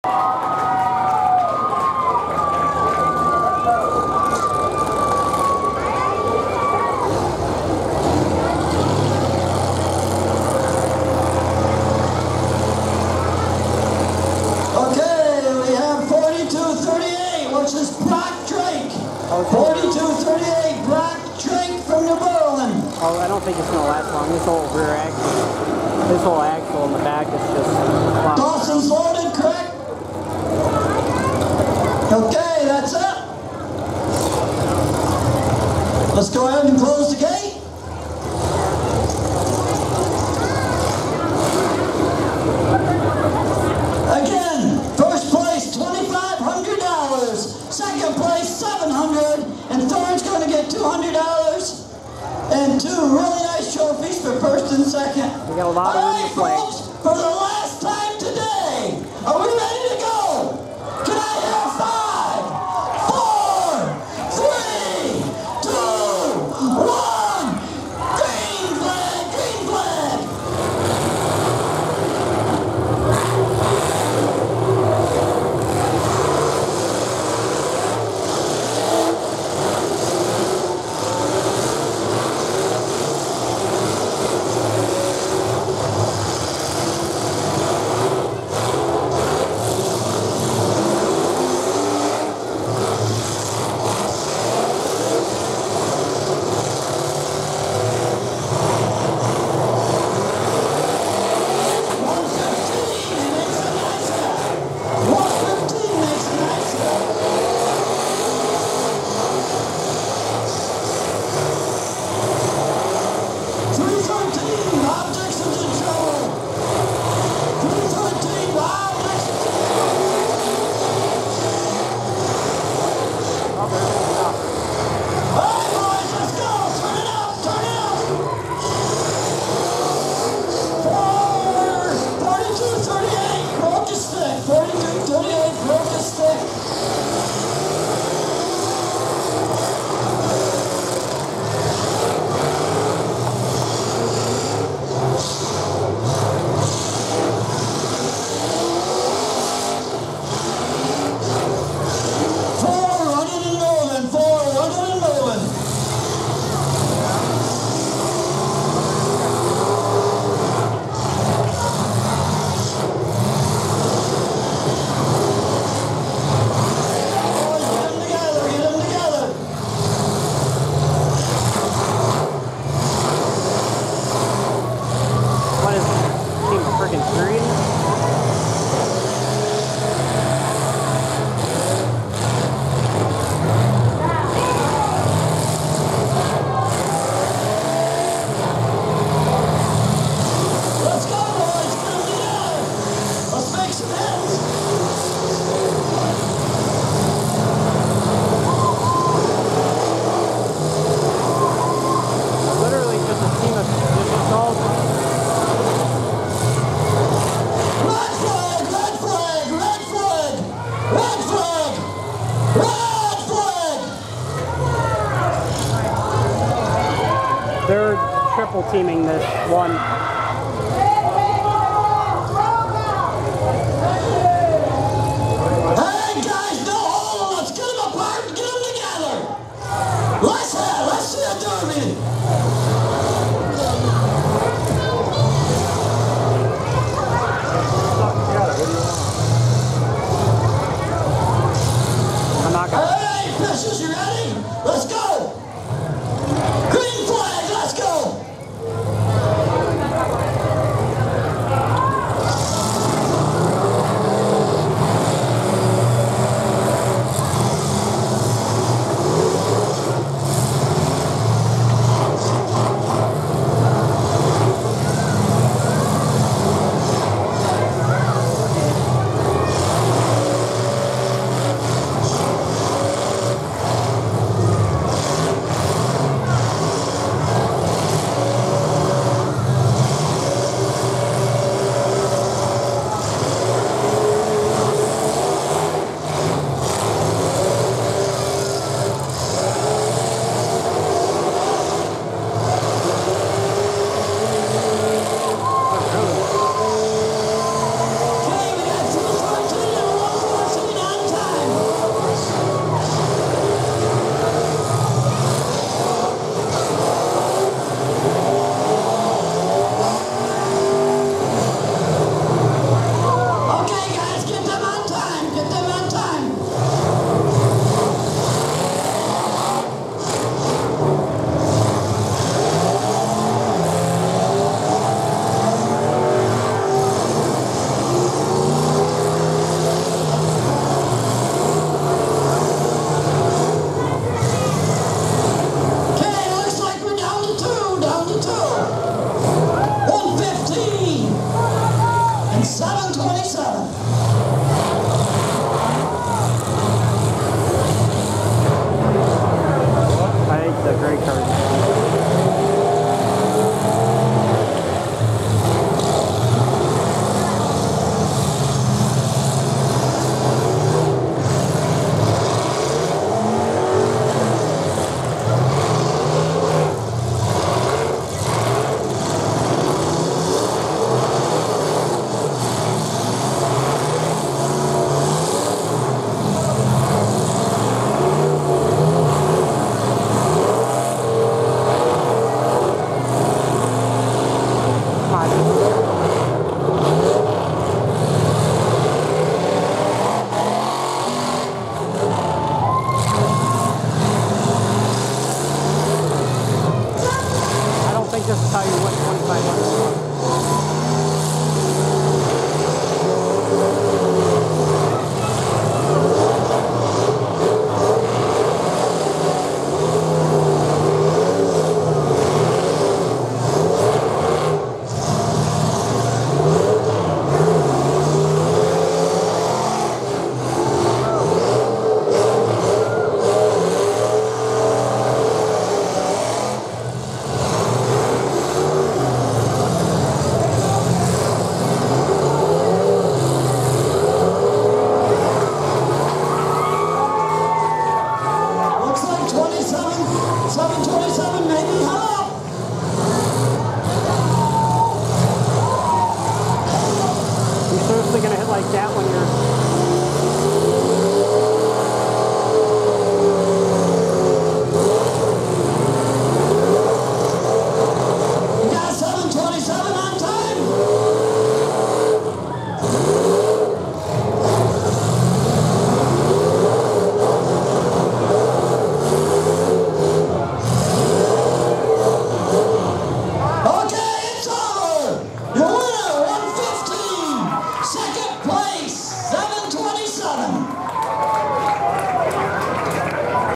2019 Madison County Fair demolition derby heat 5(Fullsize Feature)
Not a great heat, thought these guys would put on a good show. Sorry for any bad camera work the tripod was sticking.